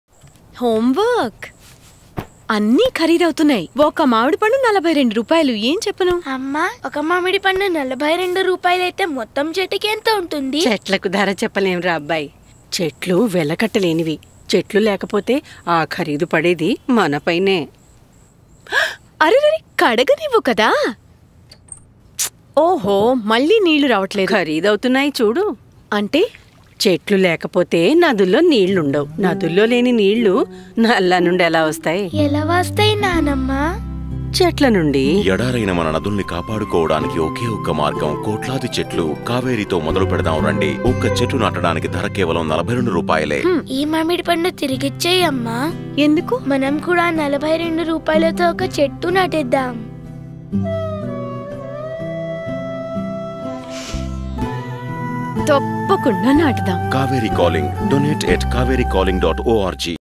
42 రూపాయలతో ఏం చెయ్యగలం?
కావేరిని రక్షించటానికి ఇప్పుడే స్పందించండి!

కావేరీ పరీవాహక ప్రాంతం ప్రపంచంలోనే అతి సారవంతమైన ప్రాంతాల్లో ఒకటి. కాని గత 70 ఏళ్లలో దానిలోని జల ప్రవాహం 40% కు పైగా పడిపోయింది. 2016 లో కావేరీ జన్మస్ధానంలో ఎండిపోయింది, వర్షపాతం 40-70% పడిపోయింది. అంతకు ముందు కొన్ని నెలల క్రితమే 2015 లో తమిళనాడు తీవ్రమైన వరదల పాలైంది. 500 వందల మంది ప్రాణాలు కోల్పోయారు. 20,000 నుంచి 160,000 కోట్ల రూపాయల మధ్య నష్టం జరిగిందని అంచనా. ఆ తరువాతి సంవత్సరం 2017 లో మళ్లీ తమిళనాడు గత 140 ఏళ్లలో లేనంత తీవ్రమైన కరవుకు గురైంది. కర్ణాటకలో ఆహార ఉత్పత్తిలో 36% లోటు చోటు చేసుకుంది. ఇలా దేశంలోని ప్రధాన నదులలో వరదలో, కరవో ఒకదాని తరువాత ఒకటి తరచుగా రావడం కనబడుతోంది. చేయవలసిన పనేదో, చేవలసిన సమయం ఆసన్నమైంది.